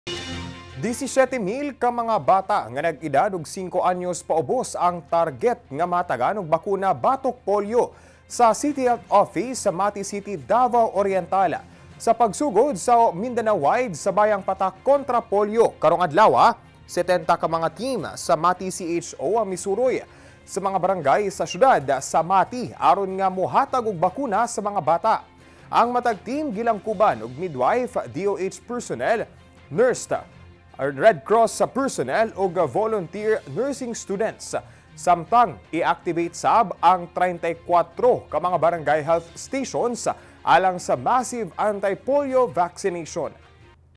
17 mil ka mga bata sa Mati City, target nga mabakunahan batok Polio
17 mil ka mga bata sa Mati City, target nga mabakunahan batok Polio